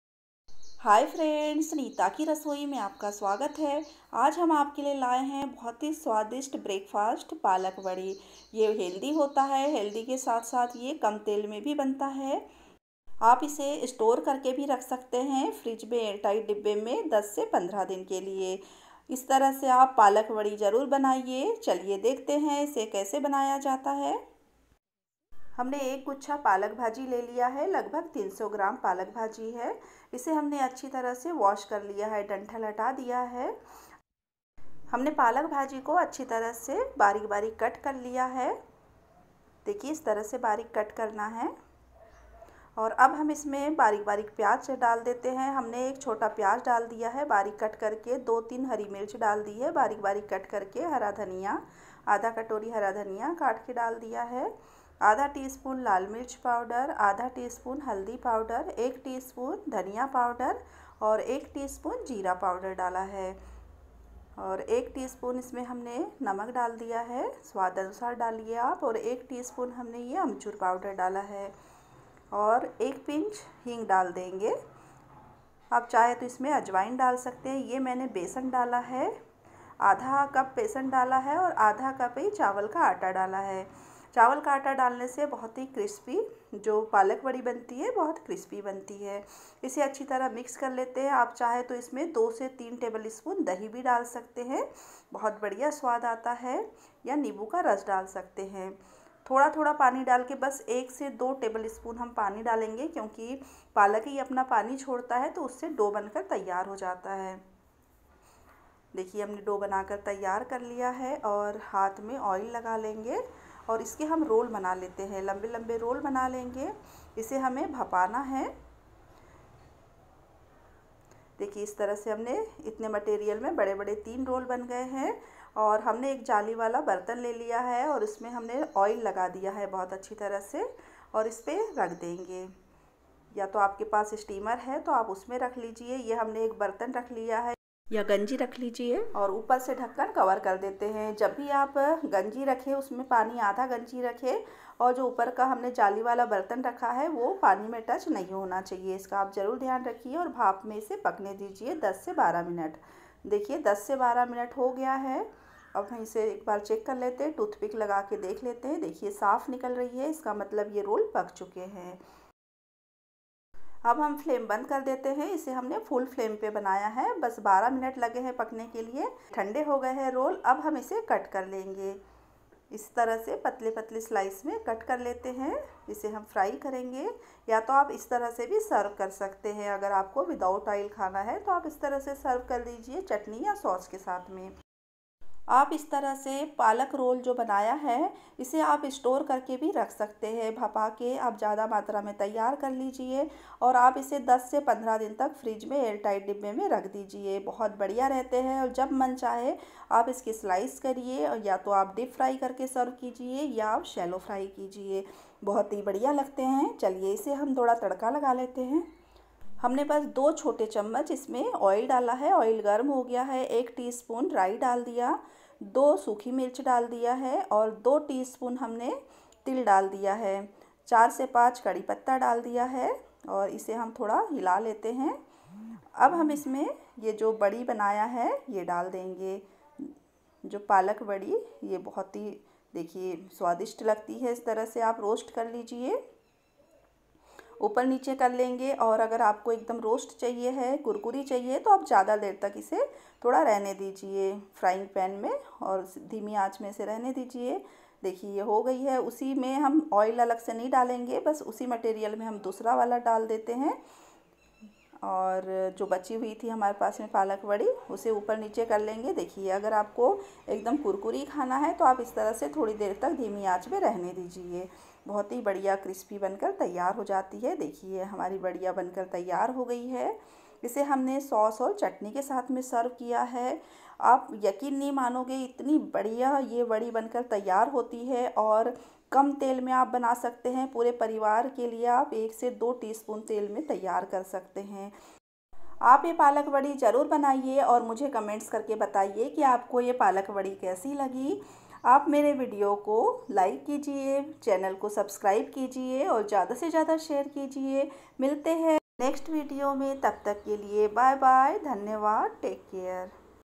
एक चम्मच तेल से बनाइए पालक का हेल्दी ब्रेकफास्ट इसे आप 10 से 15 दिन तक स्टोर करके रख सकते है
PalakVadi#healthybreakfast#palakkabreakfast#Palakbadi#Neetakirasoi#Nitakirasoi#